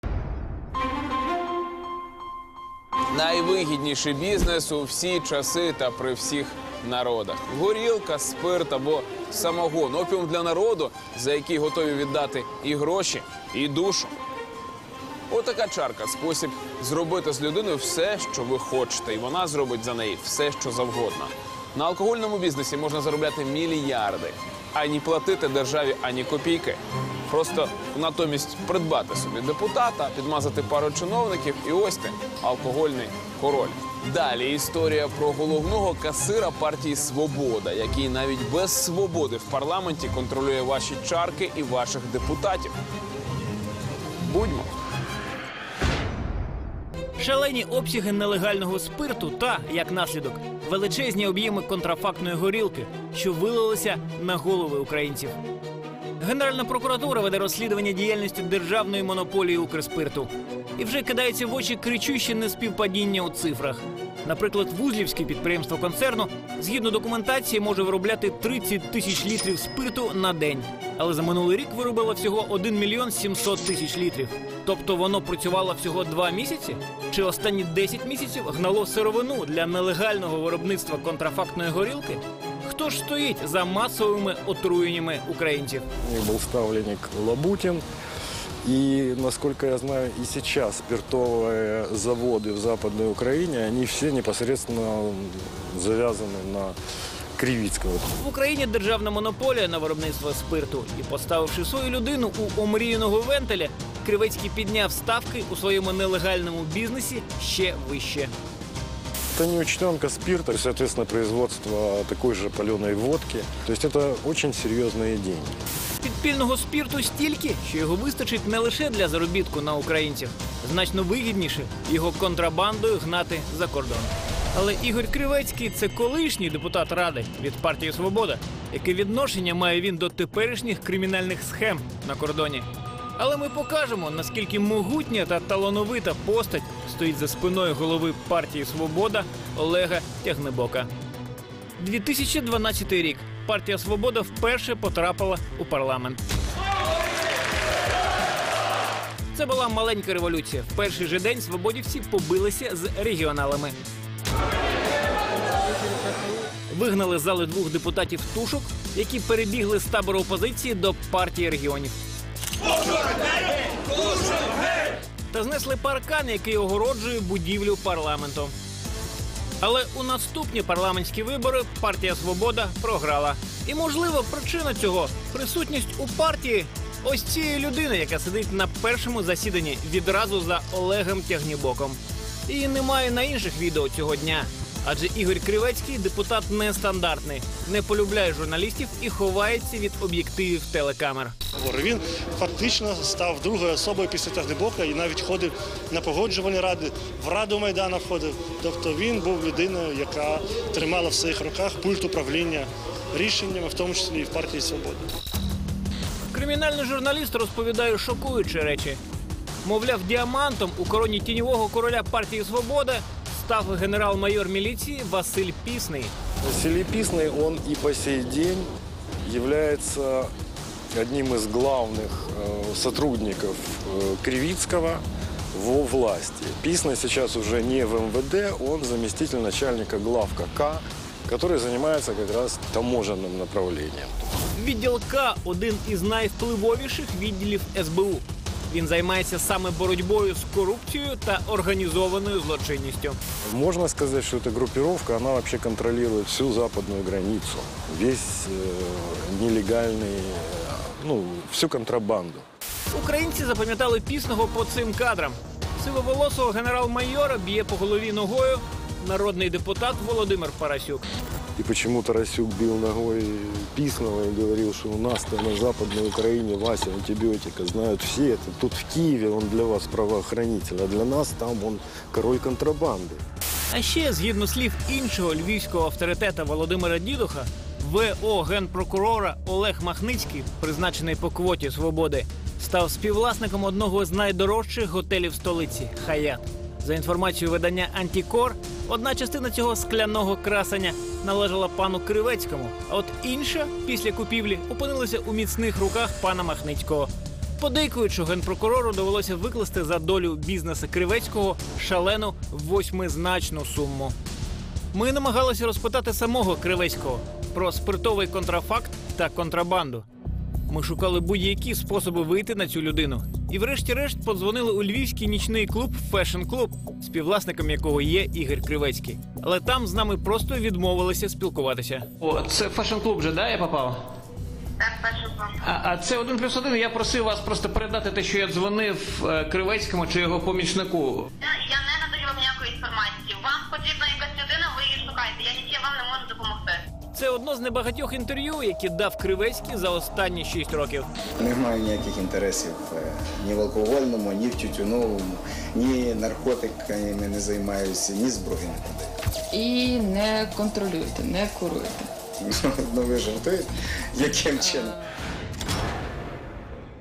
Нелегальний спирт насправді кришують у парламенті
Найвигідніший бізнес у всі часи – горілка, спирт, самогон. На алкогольному бізнесі заробляють мільярди, і при цьому не платять державі ні копійки. Ця історія – про головного касира партії «Свобода», який навіть без свободи в парламенті контролює чарки і депутатів!